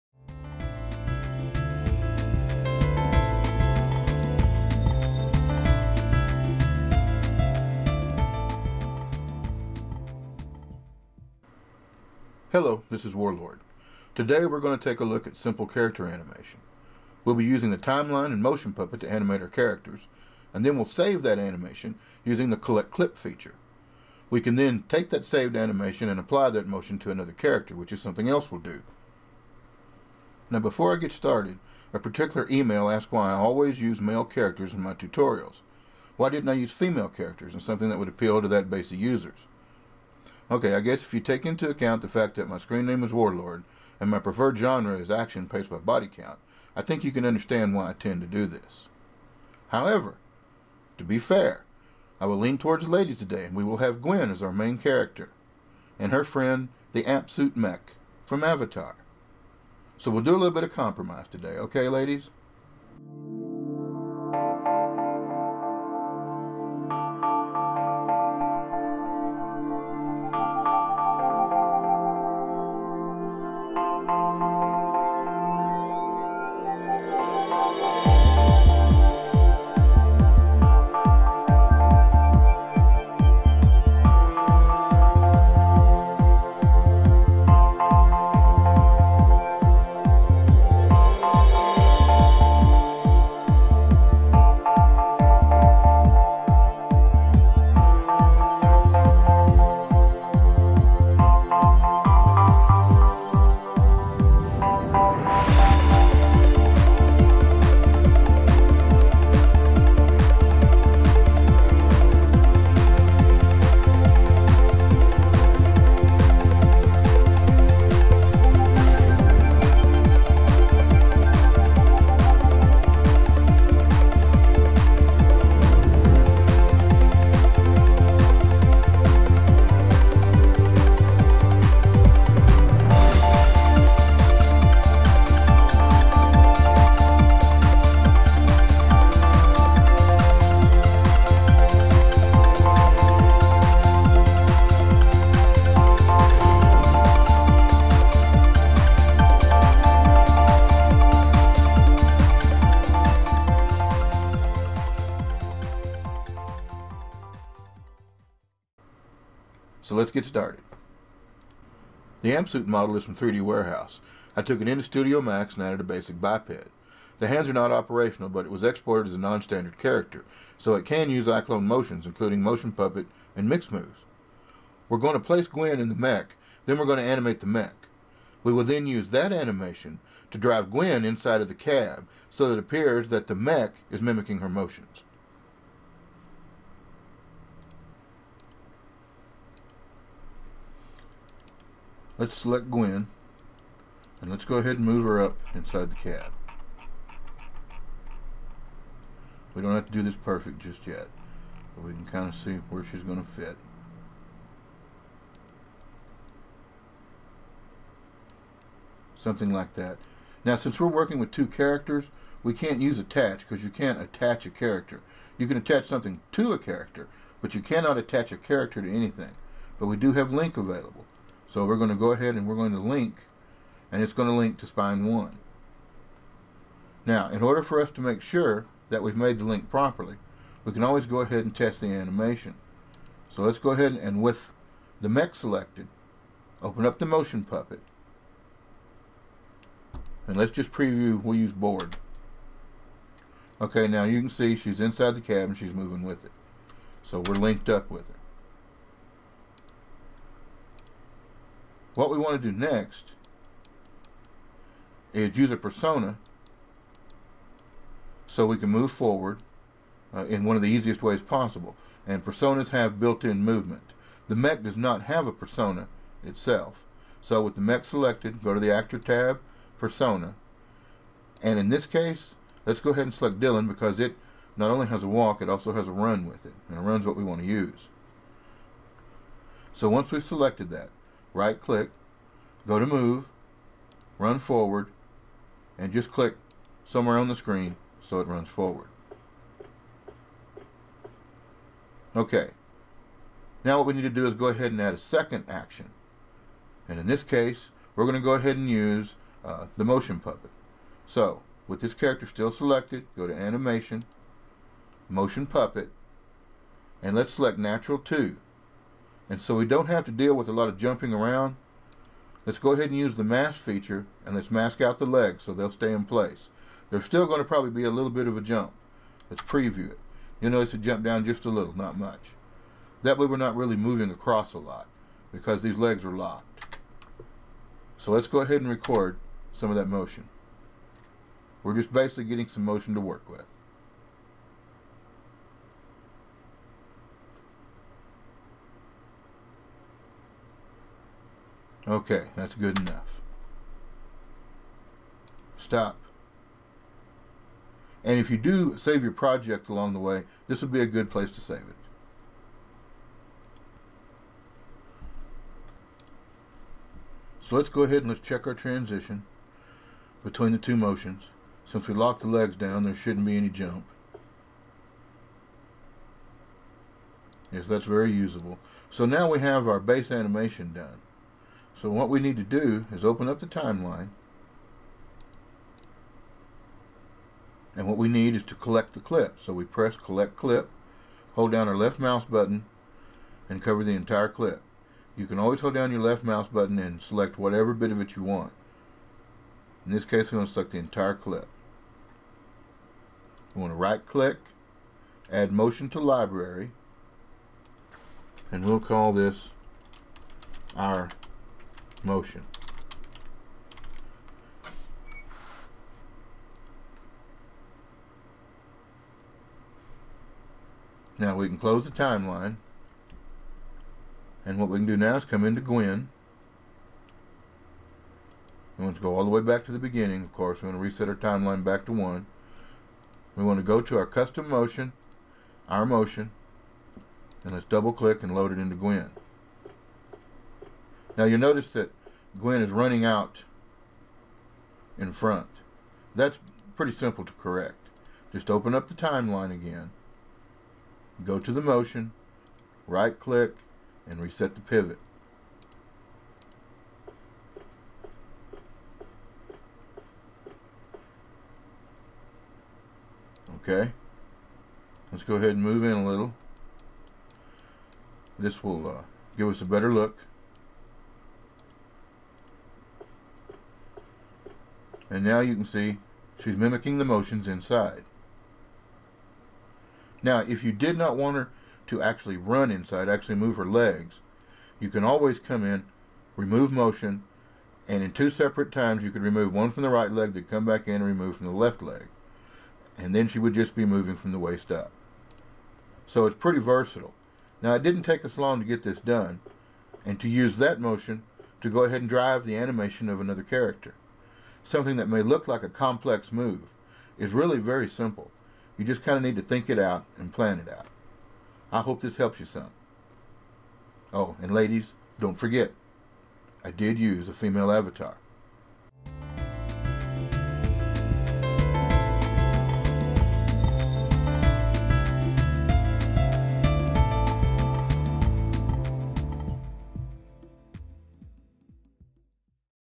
Basic Character Animation
This tutorial explores the use of collect clip to save and apply to other characters as well as showing how to work with 2 characters.

A very basic tutorial for beginners similar to the Masterchief tute but using two characters with the same motions.